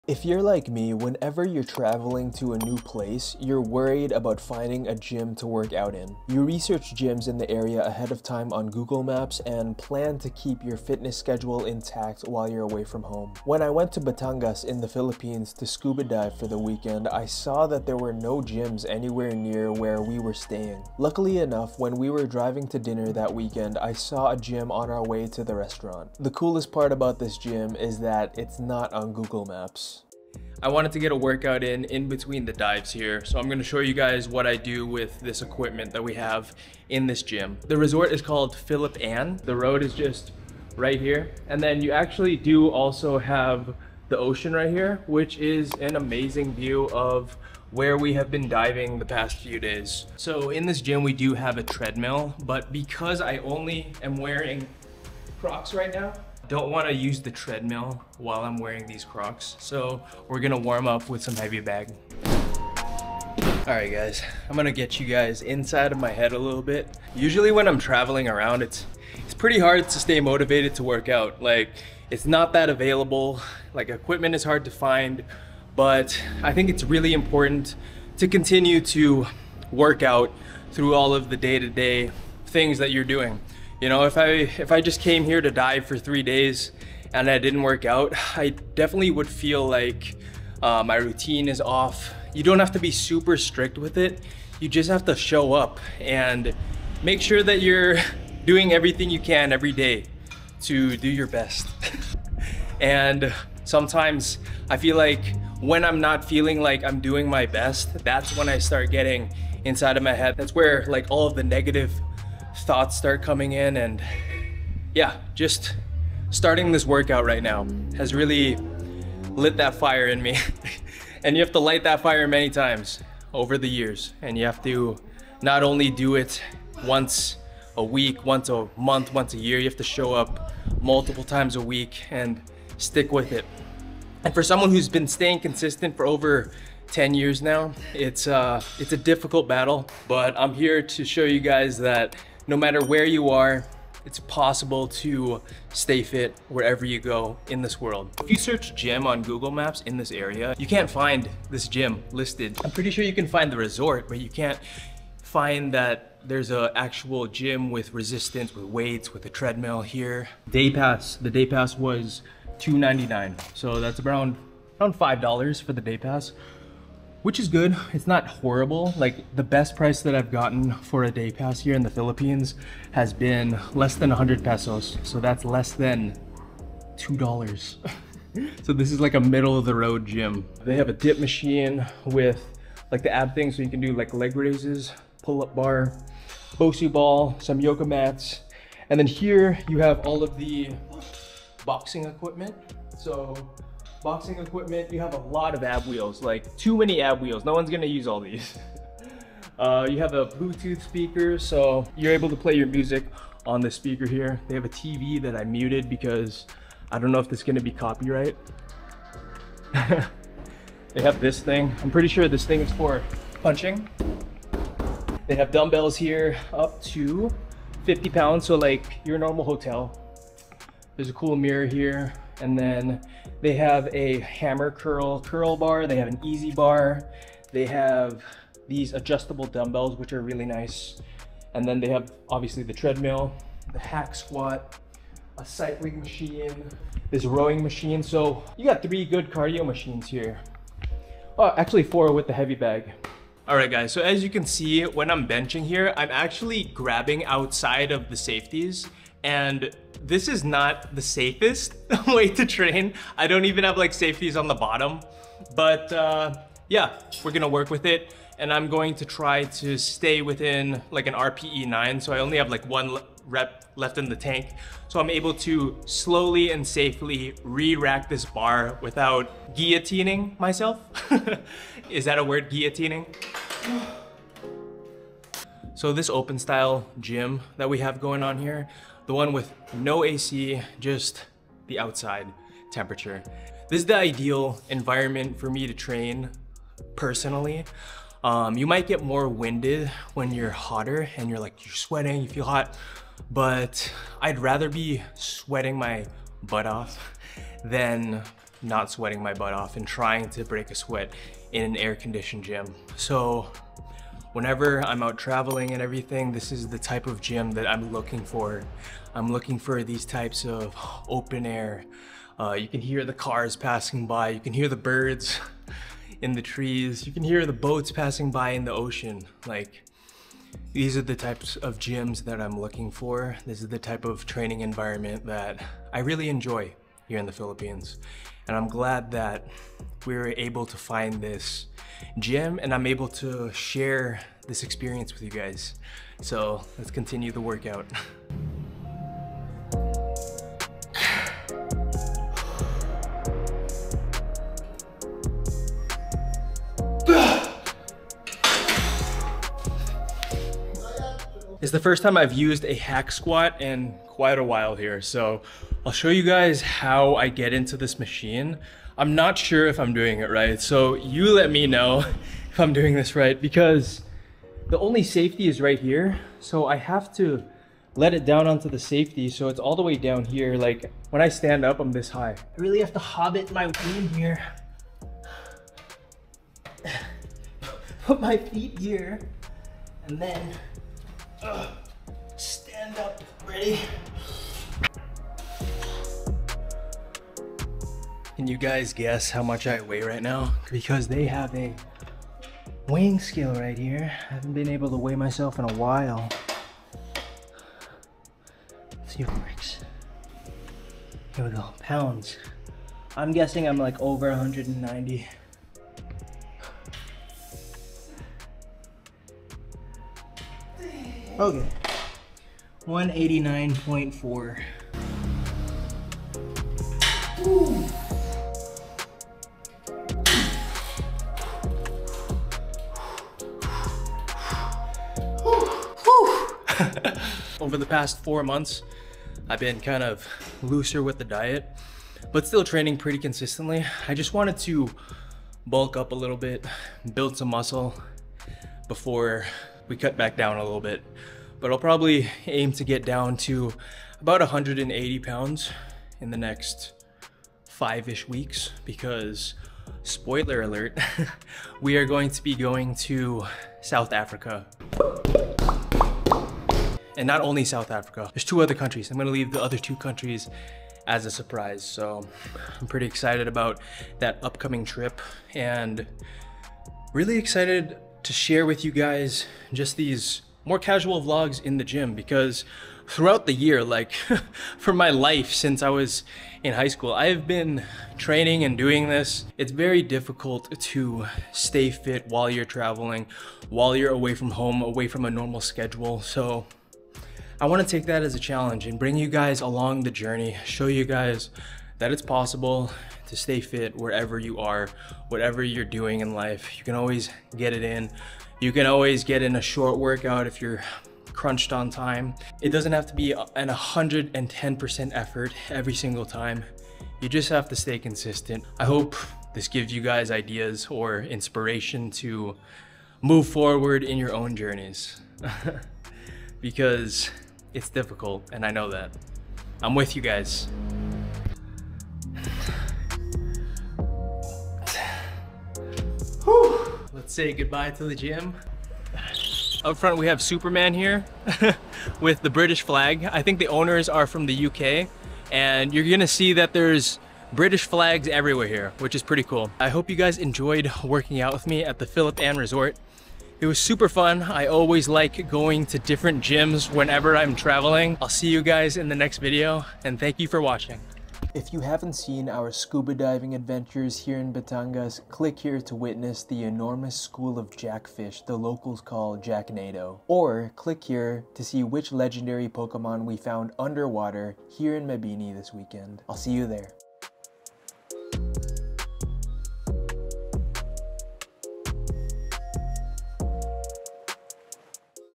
I FOUND a SECRET Gym (NOT on Google Maps)
In this video we will be working out in a hidden gym I found in Batangas, Philippines.

I was scuba diving in Batangas for the weekend, and didn't find any gyms on Google Maps before coming here.

Luckily, I found this gym and made this video.

↗️ Share the video if you think someone else can learn and enjoy this video.